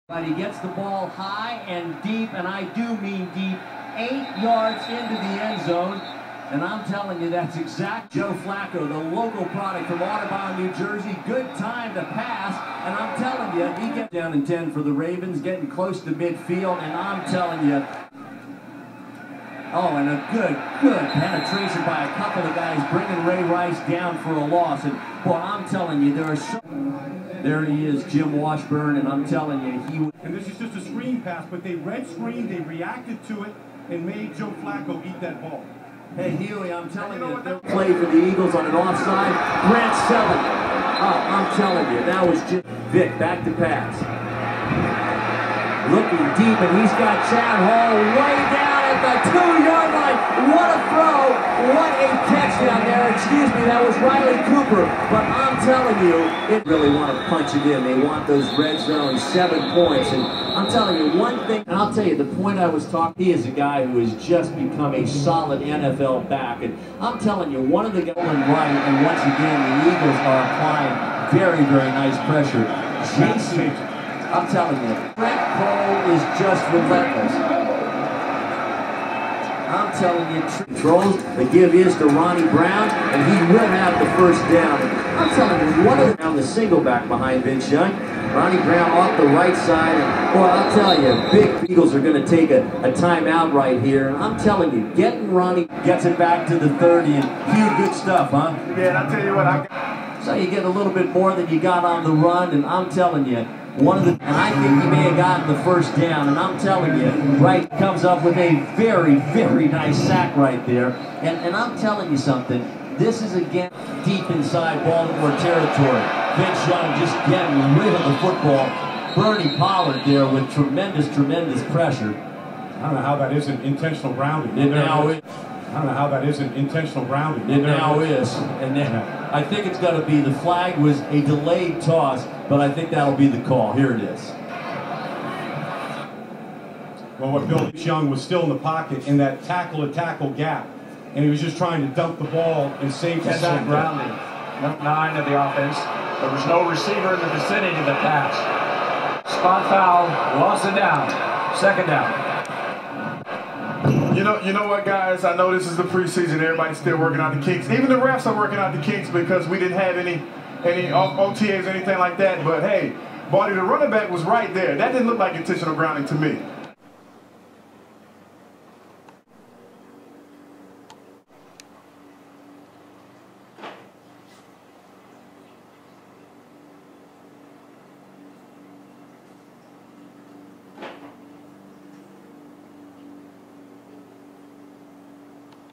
Don Tollefson
Don Tollefson is a great and enthusiastic announcer with just one funny quirk!